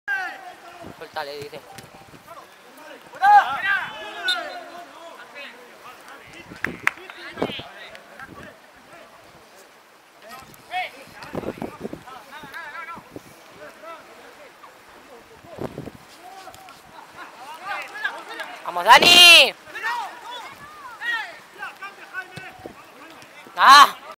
JUGADAS DE DANIEL NUEDA SABATÉ | FutbolDreams
Estas son algunas jugadas de Daniel Nueda Sabaté

CONTACTO